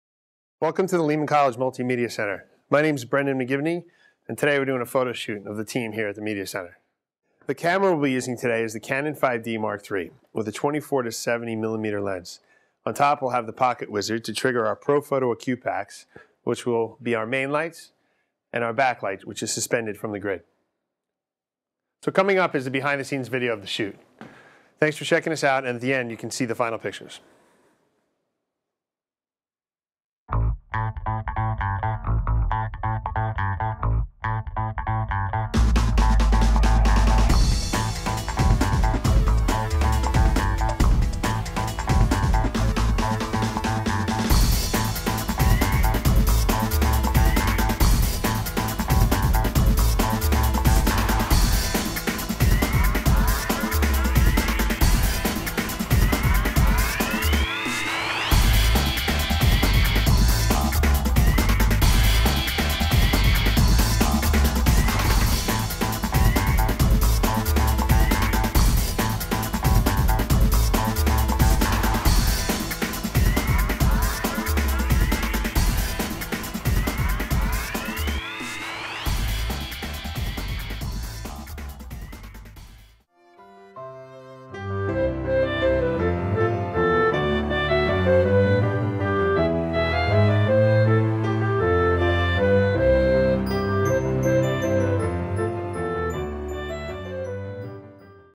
Multimedia Center Behind the Scenes - 2012 Staff Photo
(Equipment list below)

Earlier this month we brought our Multimedia Center crew together for a team photo in the studio. The whole team came together- interns, students and staff some learning, some laughs, and of course, a few photos.

Using our jib camera, we captured a little behind-the-scenes action of the setup and shoot along with a few of the final images at the end of the video as well as at the bottom of this blog. Our crew consisted of Interns Angel Ramos, Keren Minto, Josmar Taveras, Magda Soto and Brian Caraveo

Check us out, comment and enjoy! And look for more sneak peeks of us behind-the scenes in the future as we share more of what we do here at the Lehman College Multimedia Center. Happy Holidays!

Some gear info that you might interested to know more about:

- We shot stills with a Canon 5d Mark III, using a pocket wizard to trigger the strobes.

- For lighting we used a Profoto Acute 2400 with 3 soft box heads and 1 Profoto Acute B with a zoom reflector as a backlight.

- Our BTS (behind the scenes) intro and outro footage was shot on a Sony 1500 mounted on a Jimmy Jib Lite, and recorded to a Sony XD Cam